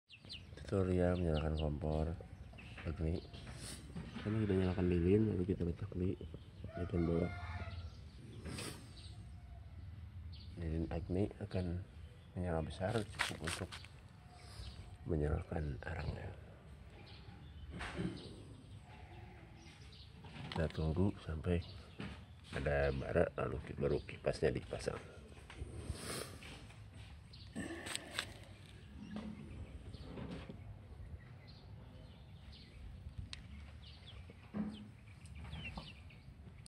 Cara menyalakan arang di Kompor Agni pakai Lilin Agni
Menyalakan arang bisa menggunakan Lilin Agni, Lilin Parafin, Gas Torch, dicelup minyak.

Mana yg mudah saja.